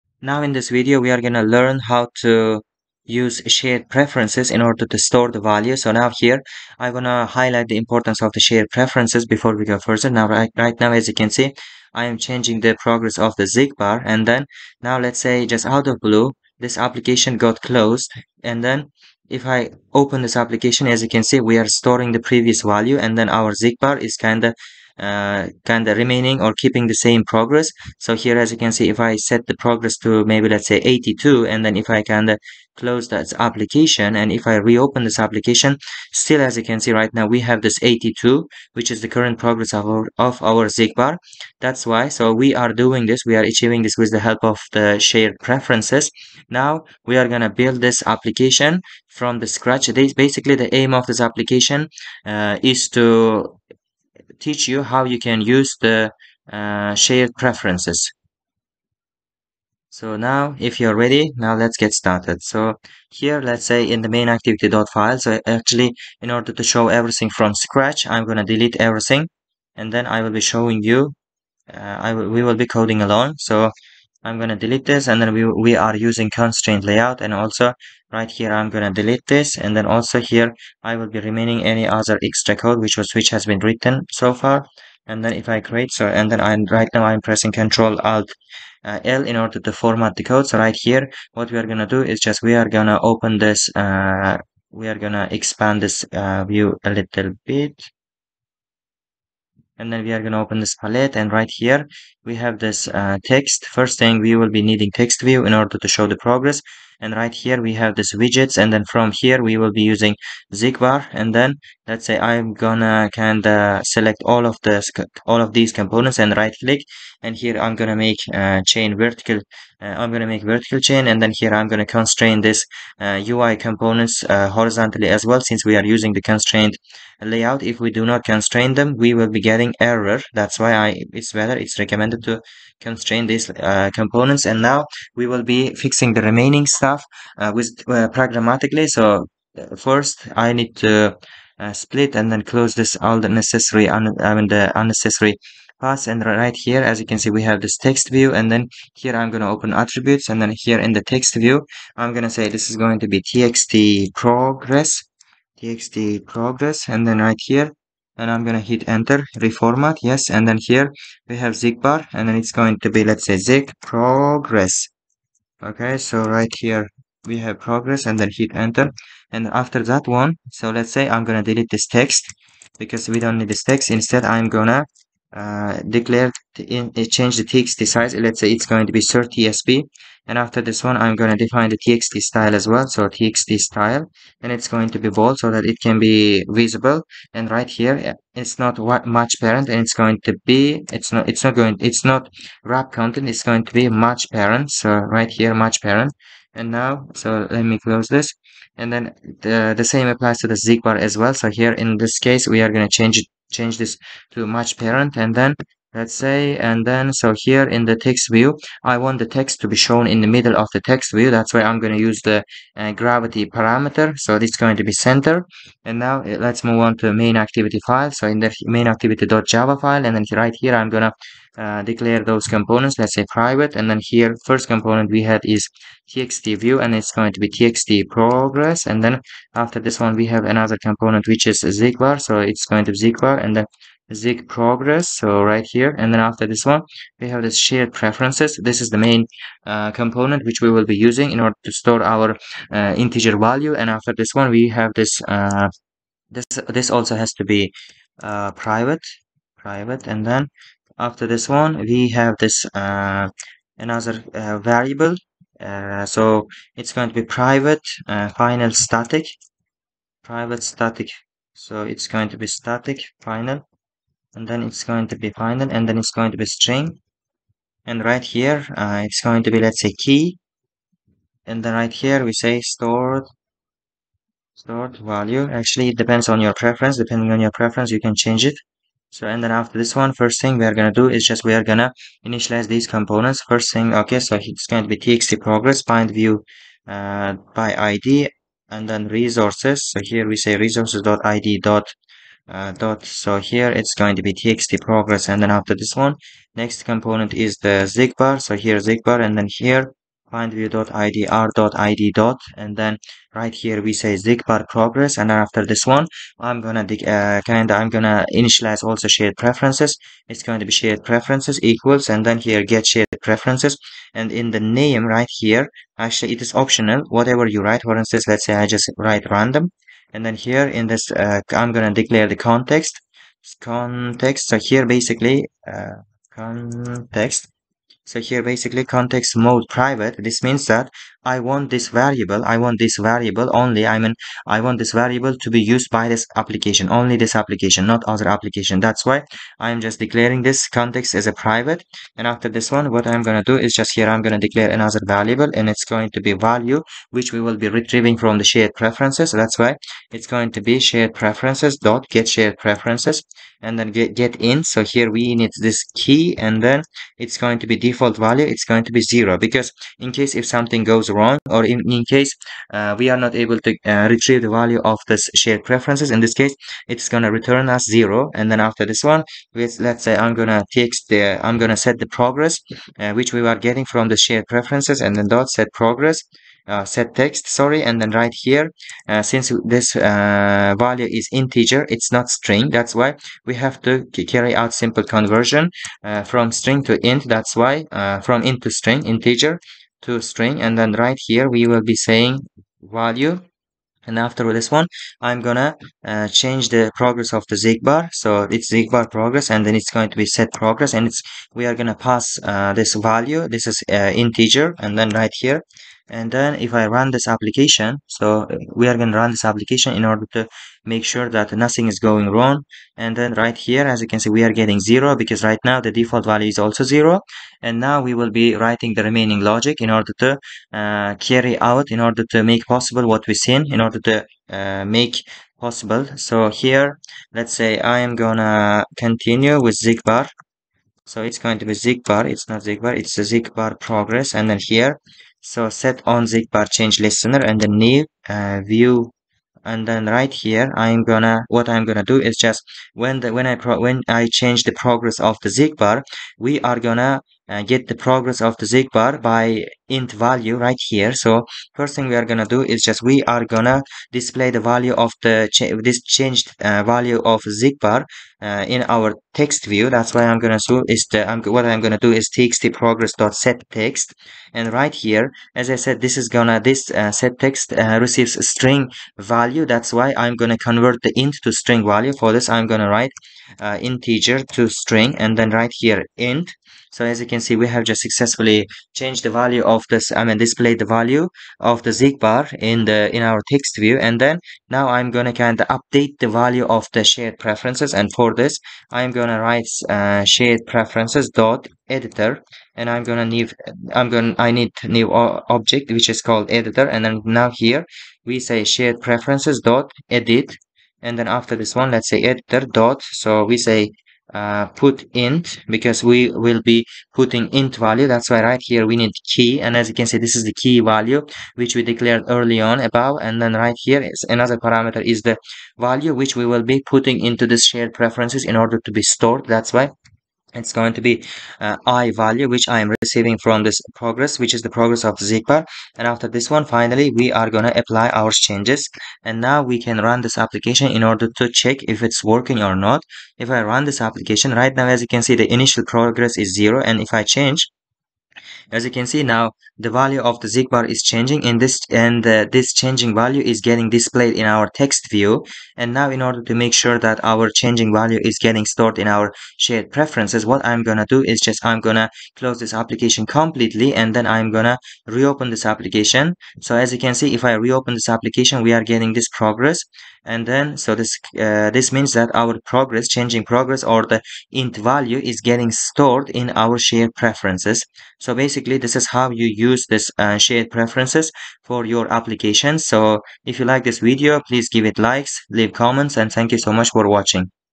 How to use shared preferences practically in android project.
Unlock the secrets of persistent data storage with our latest tutorial video! Join us as we delve into the world of Android app development, showcasing the power of using Shared Preferences to save and retrieve data within your app. In this step-by-step guide, we break down the entire process, from setting up Shared Preferences to implementing them in a simple app.

🔍 What You'll Learn:

🚀 Setting Up Shared Preferences: Discover how to configure and initialize Shared Preferences for data storage.
💾 Saving and Retrieving Data: Learn the art of efficiently storing and accessing data in your Android app.
🌐 Practical Usage: See how Shared Preferences enhance user experience by saving preferences, settings, and more.
📱 Hands-On Tutorial: Follow along as we demonstrate each step using a simple real-world application.
Whether you're a novice or an experienced developer, this tutorial is your gateway to mastering Shared Preferences in your Android apps. Enhance your skills and create apps that remember user preferences seamlessly.